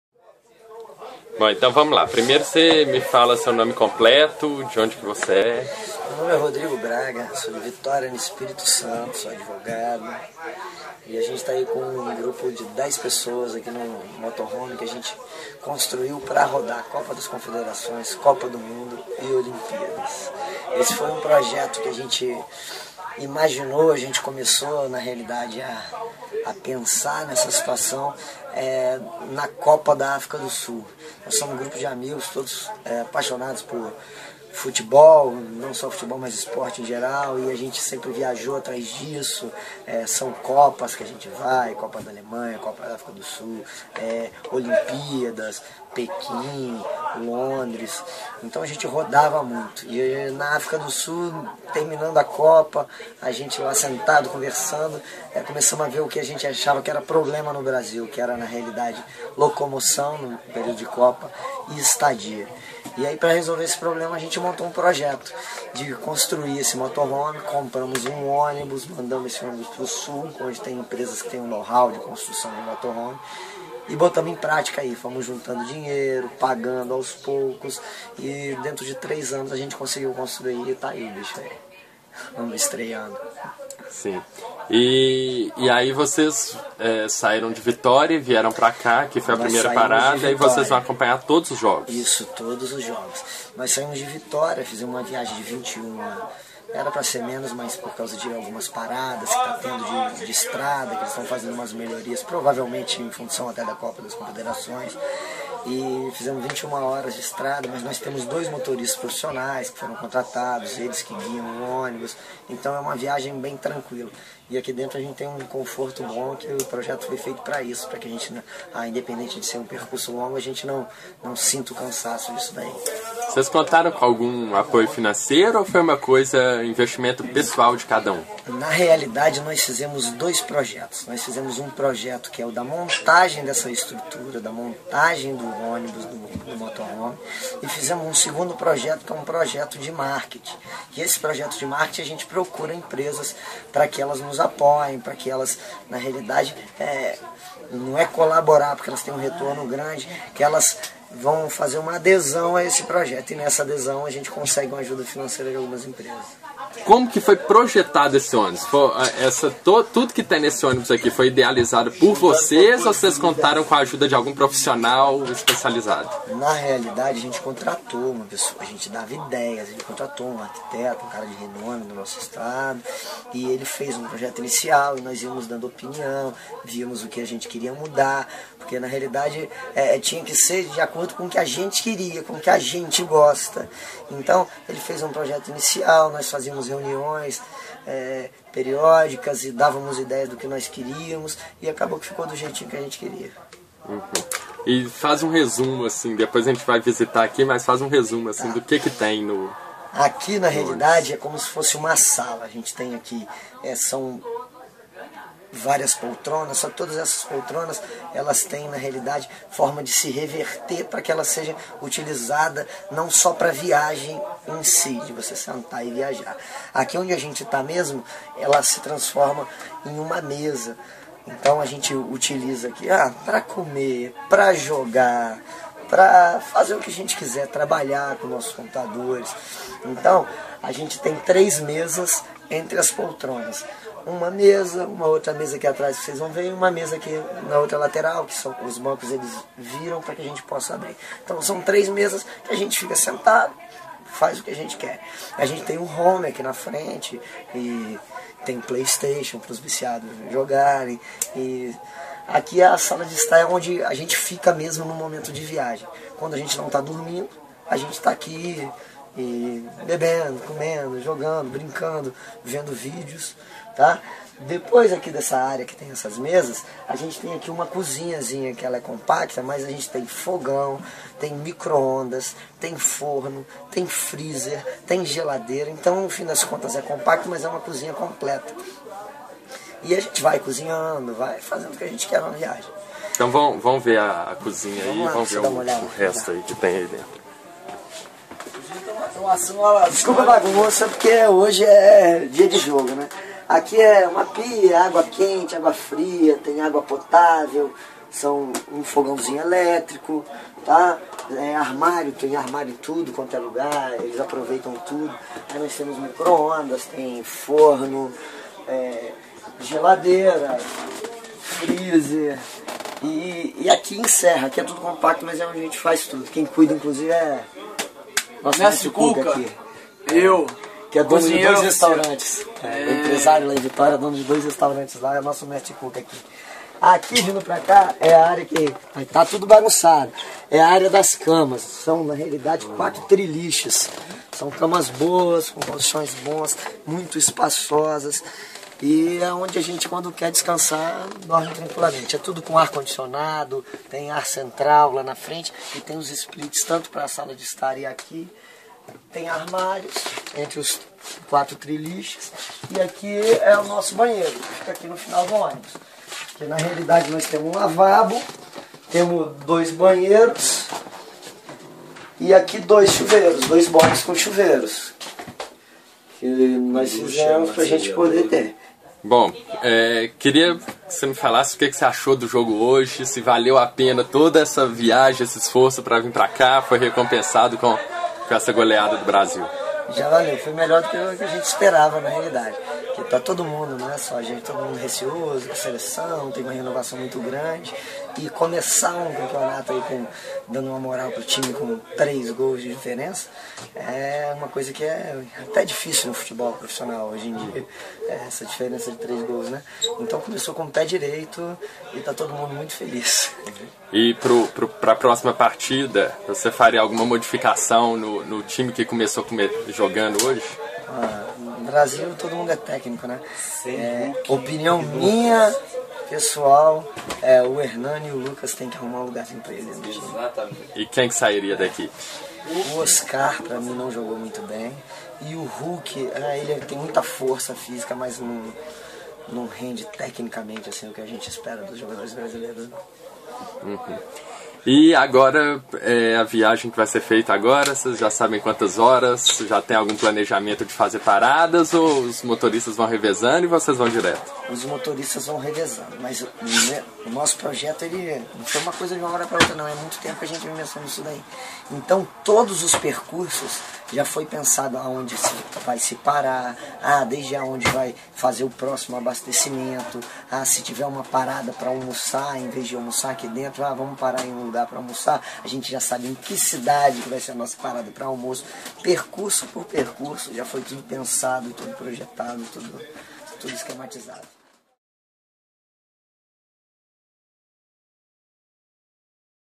Integrantes do projeto "Torcida na Estrada" seguem de ônibus para cidades que receberão a Seleção
Confira a entrevista com Rodrigo Braga e conheça o motorhome onde 10 amigos viverão durante a Copa das Confederações